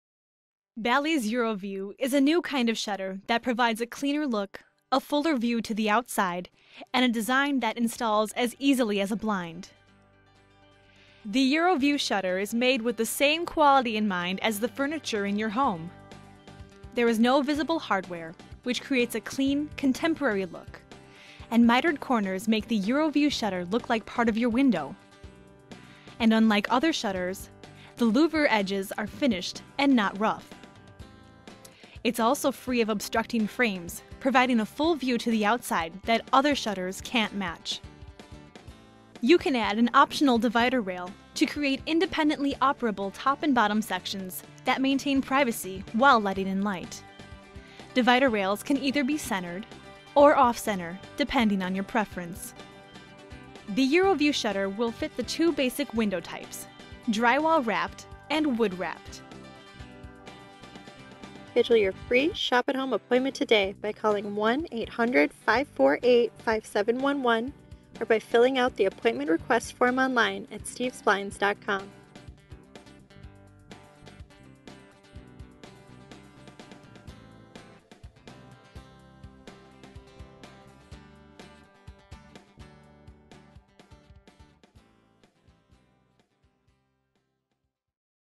Bali's EuroVue Shutters Brought to You by Steve's Blinds & Wallpaper!
Bali's EuroVue Shutter offers a cleaner look, a fuller view to the outside and a design that installs as easily as a blind. A clean, contemporary design with no visible hardware and mitered corners makes the shutter look like part of your window. It's also free of obstructing frames, so just opening the louvers provides a fuller view to the outside.

    • Available with your choice of four distinctive panel edge designs.
    • Mortise and tendon joinery ensures long-lasting durability.
    • To install, simply attach the hinge mounting plates and snap the shutter into place.
    • Installs easily on drywall-wrapped and wood-wrapped windows.

Purchase your Bali shutters at Steve's Blinds & Wallpaper and you will always save 25% to 85% off retail and home center store prices! Shop now!